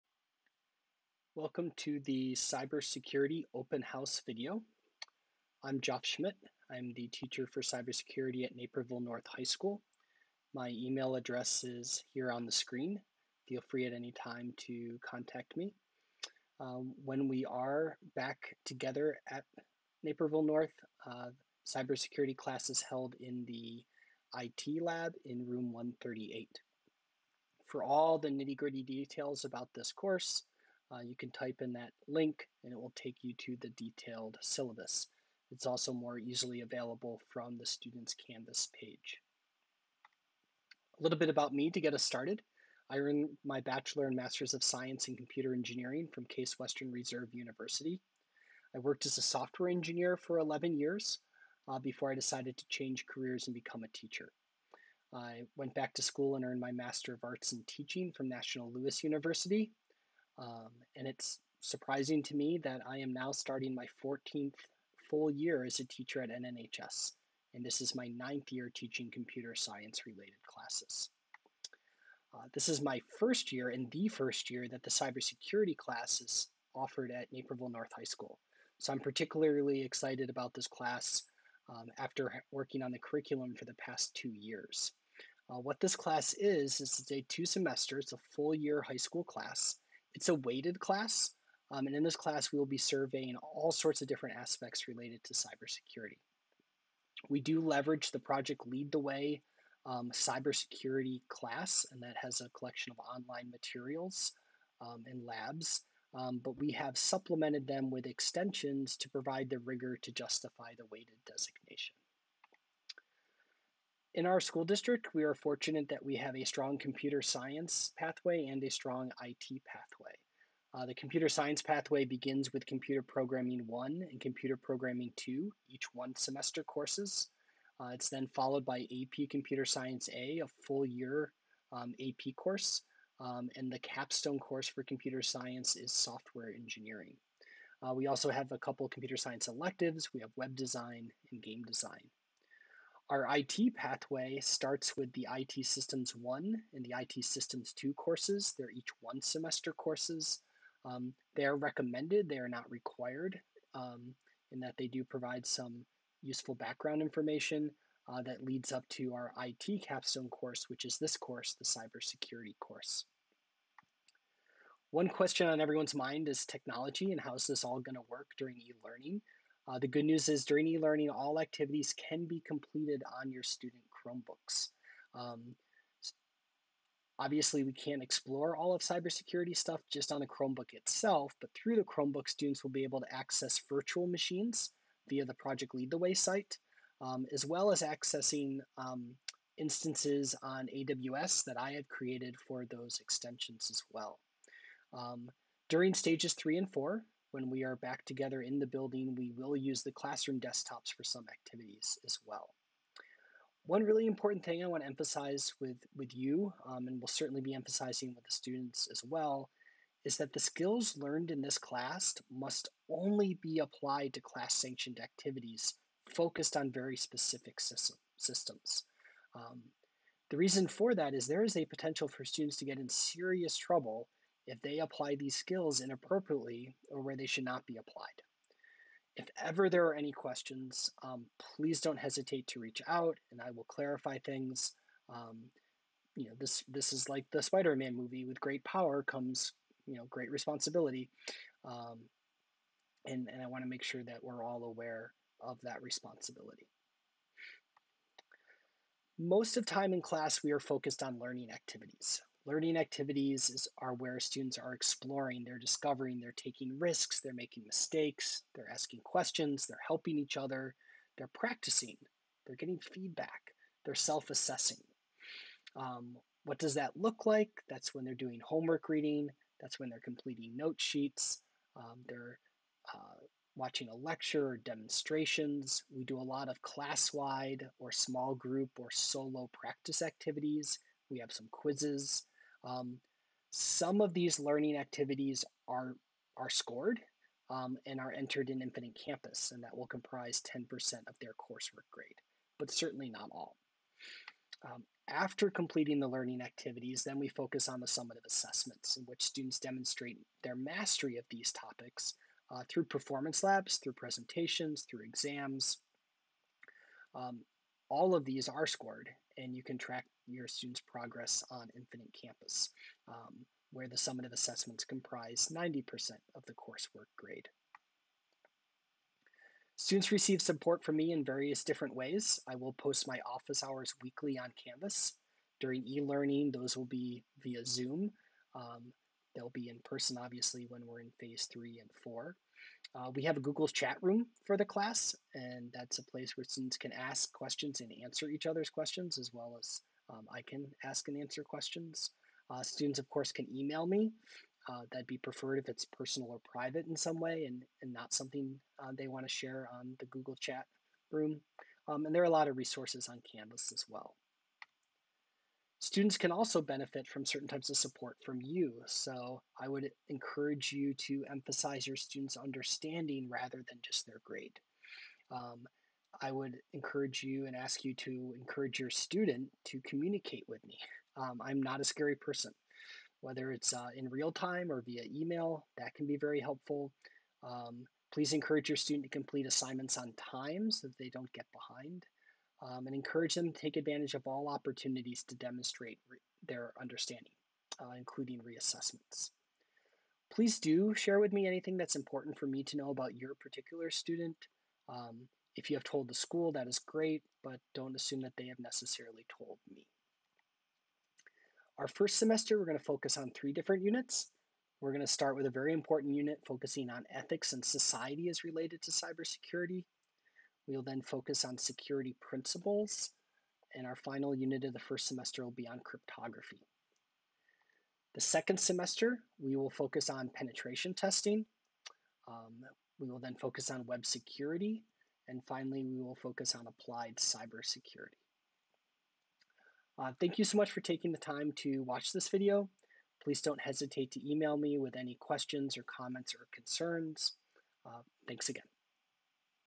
Open House Video for Cybersecurity
The open house video for Cybersecurity for the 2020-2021 school year at Naperville North High School.